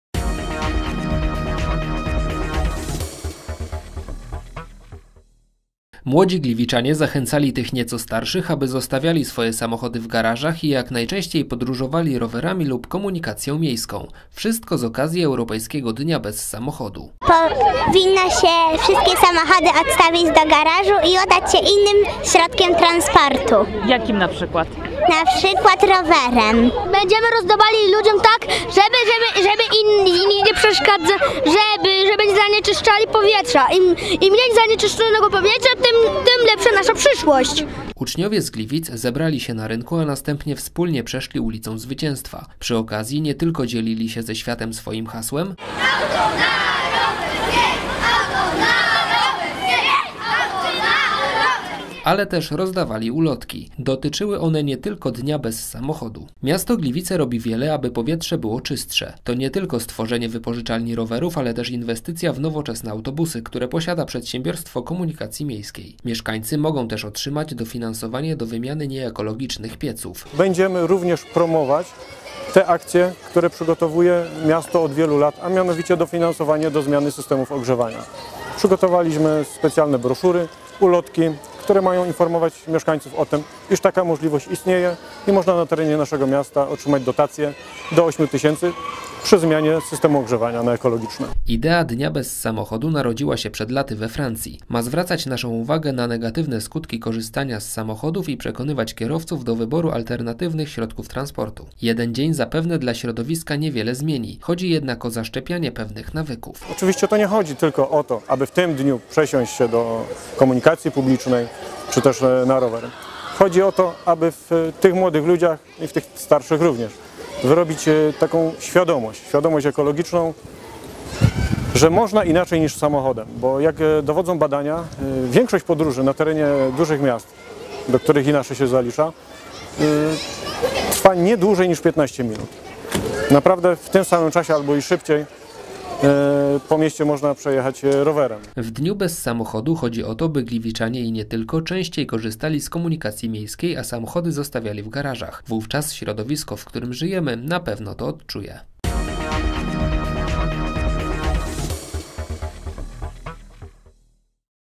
A może choć 1 dzień bez samochodu?
Najmłodsi gliwiczanie zachęcali tych nieco starszych, aby zostawiali swoje samochody w garażach i jak najczęściej podróżowali rowerami lub komunikacją miejską. Wszystko z okazji Europejskiego Dnia bez Samochodu.